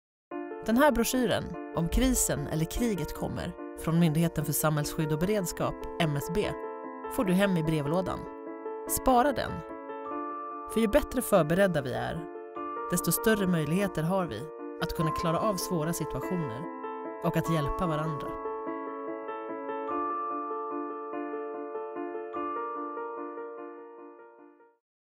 Broschyren "Om krisen eller kriget kommer"
En informationsfilm om broschyren "Om krisen eller kriget kommer" som skickas till alla svenska hushåll under vecka 22. Den kommer från Myndigheten för samhällsskydd och beredskap (MSB) och ska hjälpa oss att bli bättre förberedda på allt från allvarliga olyckor, extremt väder och it-attacker till militära konflikter. Spara broschyren!